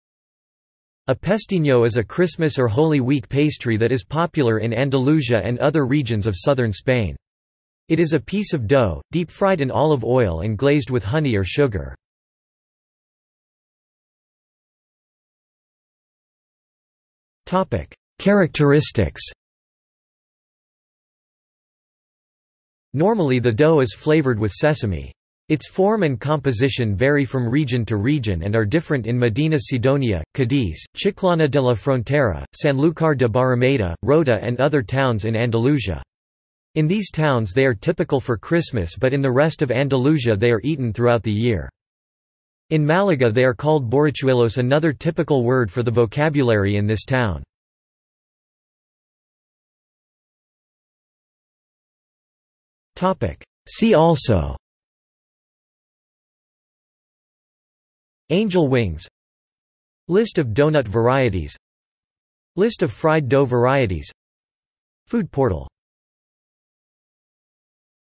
Pestiños | Wikipedia audio article
This is an audio version of the Wikipedia Article:
Pestiños

00:00:13 1 Characteristics
00:00:49 2 See also

- Socrates

SUMMARY
A pestiño is a Christmas or Holy Week pastry that is popular in Andalusia and other regions of Southern Spain. It is a piece of dough, deep fried in olive oil and glazed with honey or sugar.